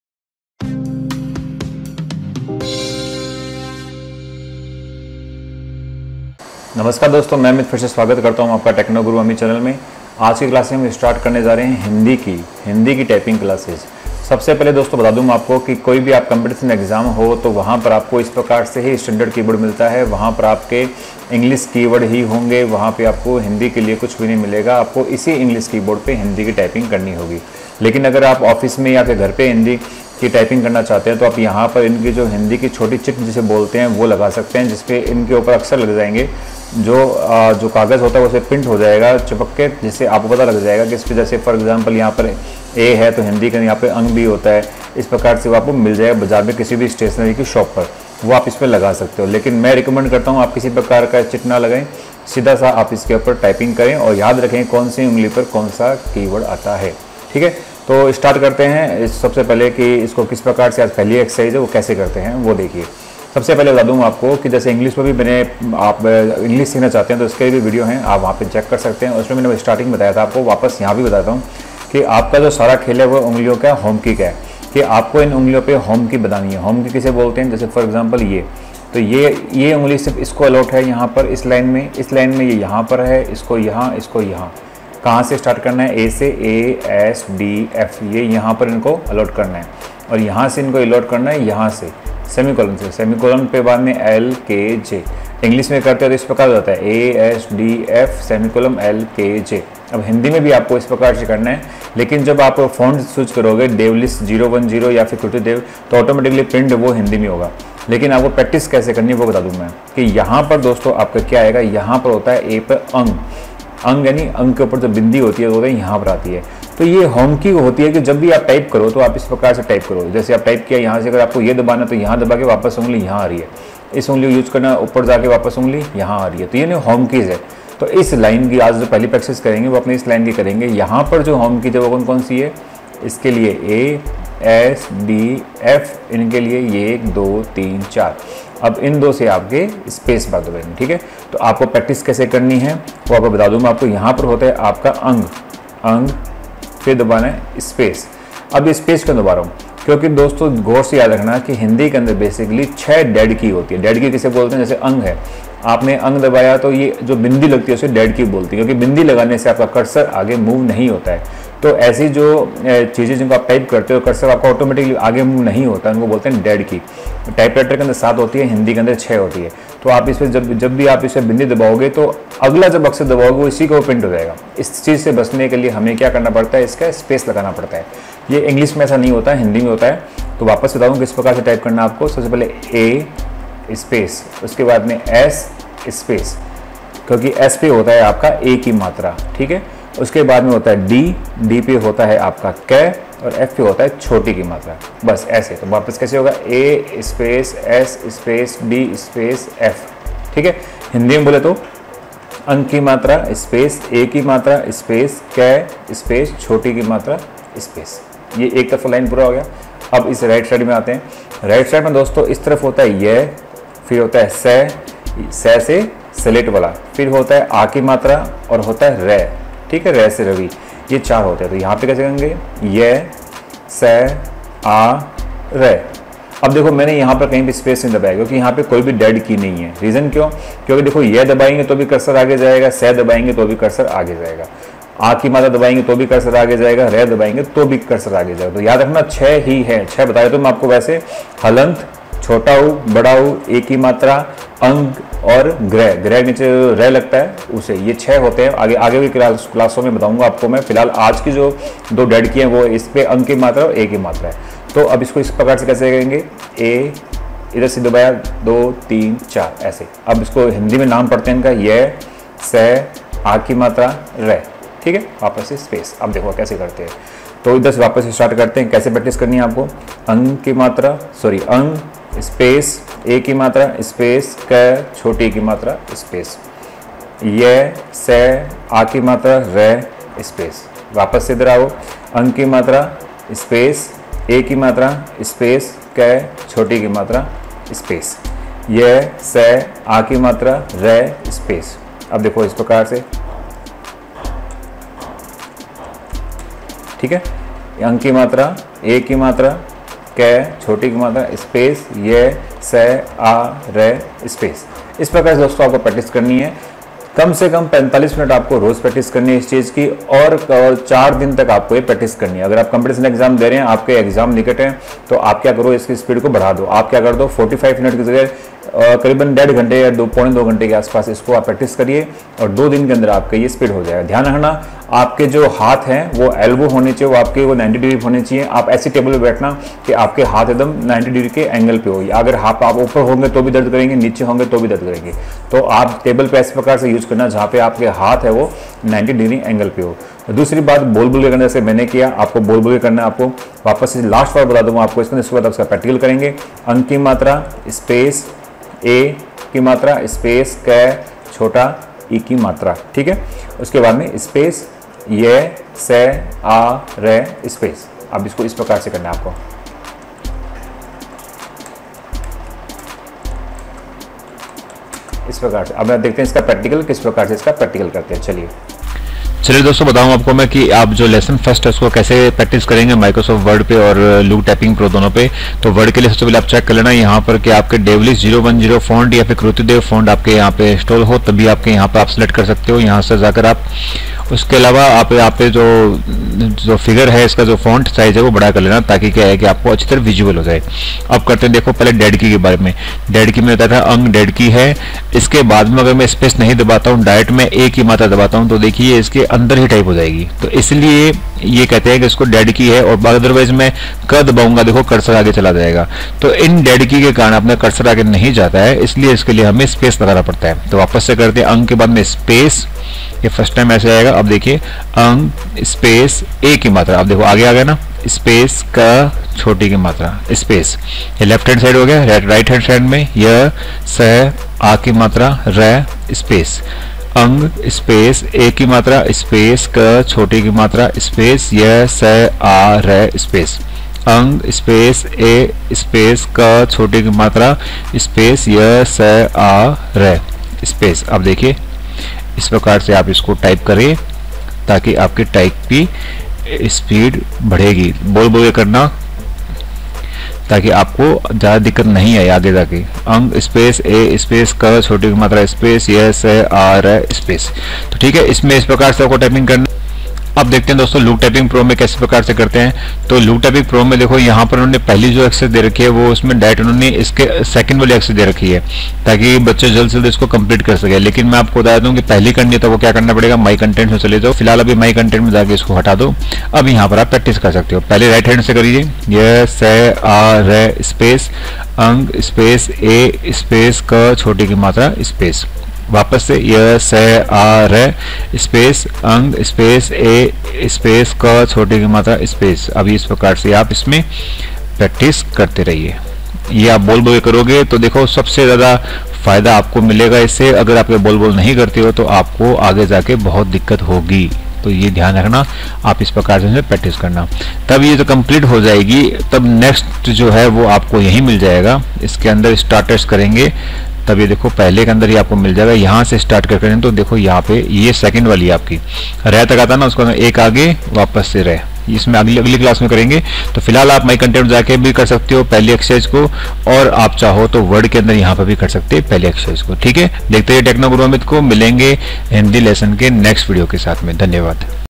How to learn Hindi typing at home | Lesson 1
How to learn Hindi typing at home | part 1
Online Hindi Typing Tutor.
Yes Here is free hindi typing classes for competition exam like(IA exam 2018,clerk grade,banks,ssc,panchayats etc.). so learn hindi typing course just in 1 month and you will get more than 35 wpm speed.
Agar aapko accuracy bhi check karni hai then download "techno Typing Pro software".

:Best Keyboards for Typing:

HP Keyboard
Logitech Keyboard

Tech Tutorials and Computer Classes
Purchase for life time registration